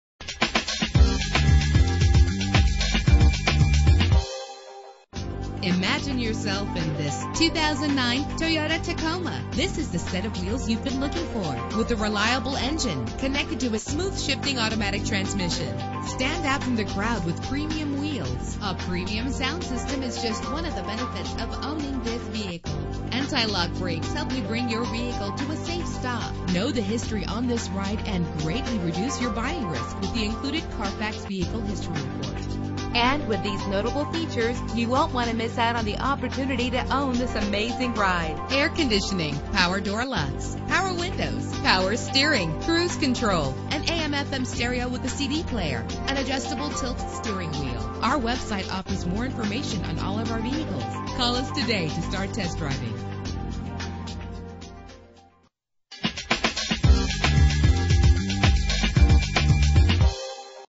2009 Toyota Tacoma - MESA AZ
Year: 2009
Make: Toyota
Model: Tacoma
Trim: PreRunner SR5 4D Double-Cab Truck
Engine: 4.0 Liter
Transmission: Automatic
Color: White
Mileage: 28428
Address:
6136 E AUTO LOOP AVE